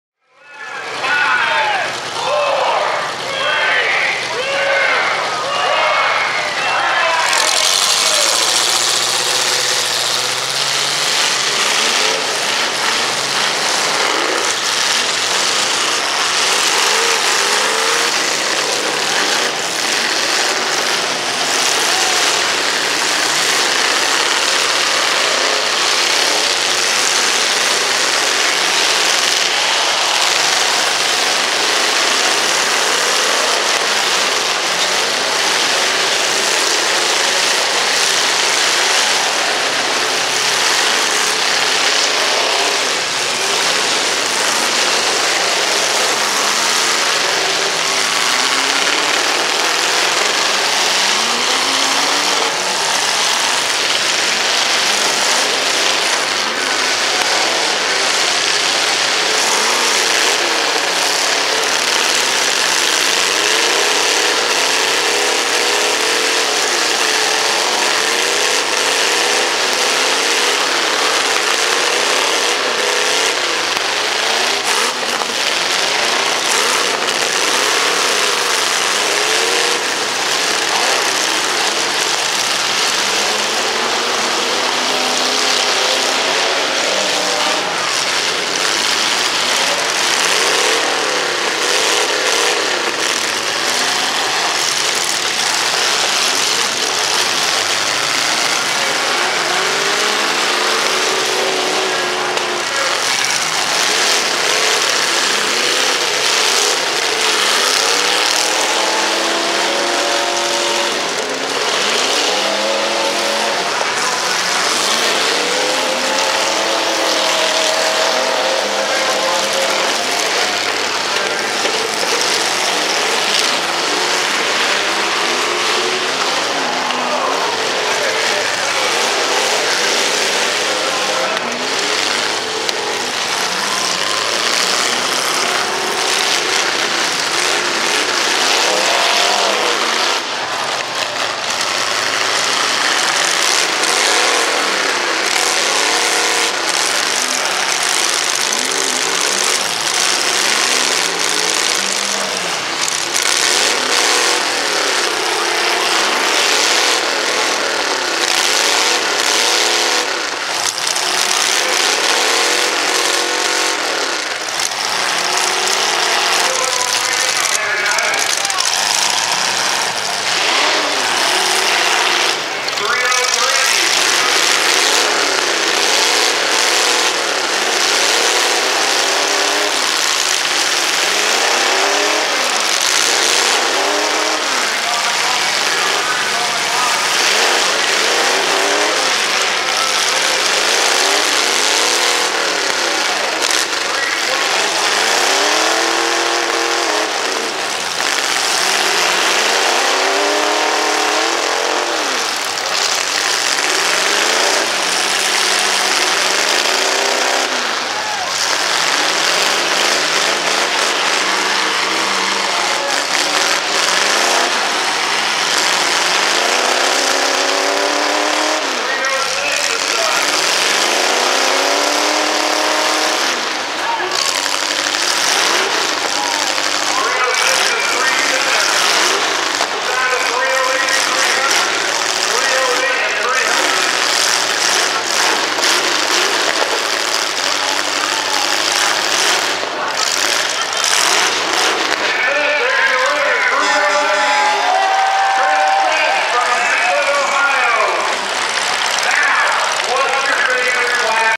Fullsize Trucks - Defiance County Derby 2019
Thanks to our friend Toby for filming on our behalf. Video footage of the 2019 Defiance County Fair Derby. Took place in Hicksville Ohio on August 24th.